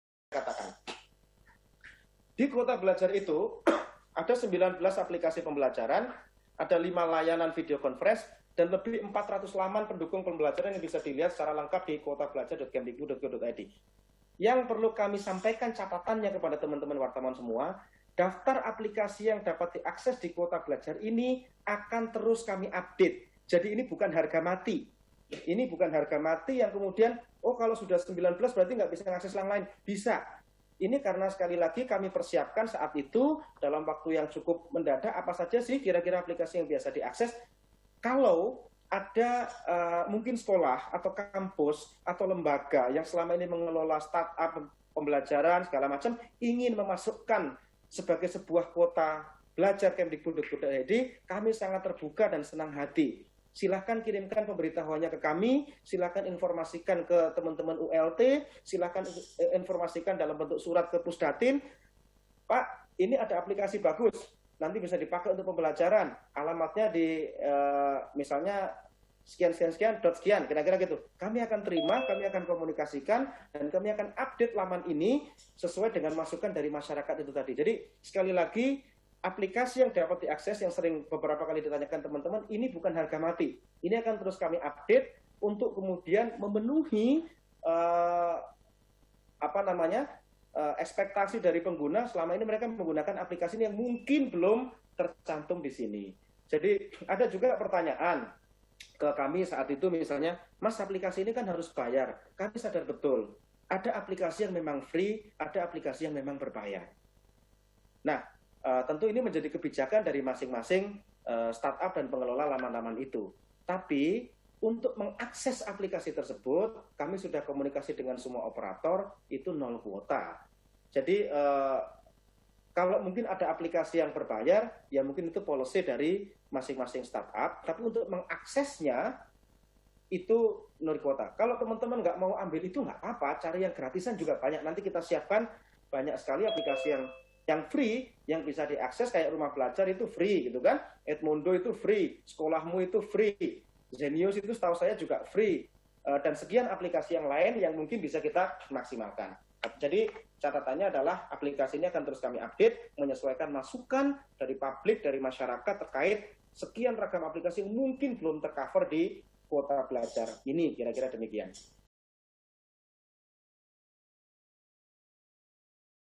Daftar Aplikasi dan Laman Web yang Dapat Diakses Menggunakan Kuota Internet Kemdikbud
JAKARTA, KOMPAS.TV - Pelaksana Tugas (PLT) Kepala Pusat Data dan Informasi (Pusdatin) Kementerian Pendidikan dan Kebudayaan (Kemdikbud) Muhammad Hasan Chabibie S.T., M.Si., memberikan daftar aplikasi dan laman web yang bisa diakses menggunakan bantuan kuota internet Kemdikbud.

PLT Kapusdatin menyampaikan hal tersebut dalam video conference dengan aplikasi Zoom pada hari Selasa (29/9/2020).

Video conference diadakan secara terbuka dan dihadiri oleh banyak pewarta media.

PLT Kapusdatin menyebutkan ada 19 aplikasi, 5 layanan video conference, dan 400 laman web yang dapat diakses menggunakan kuota internet bantuan dari Kemdikbud.

19 Aplikasi tersebut adalah:

1. Aminin,

2. Kipin School 4.0,

3. AyoBlajar,

4. Microsoft Education,

5. Bahaso,

6. Quipper,

7. Birru,

8. Ruang Guru,

9. Cakap,

10. Rumah Belajar,

11. Duolingo,

12. Sekolah.Mu,

13. Edmodo,

14. Udemi,

15. Eduka System,

16. Zenius,

17. Ganeca Digital,

18. Whatsapp,

19. Google Classroom.

5 Layanan Video Conference:

1. Cisco Webex,

2. Google Meet,

3. Microsoft Team,

4. Zoom,

5. U Meet Me.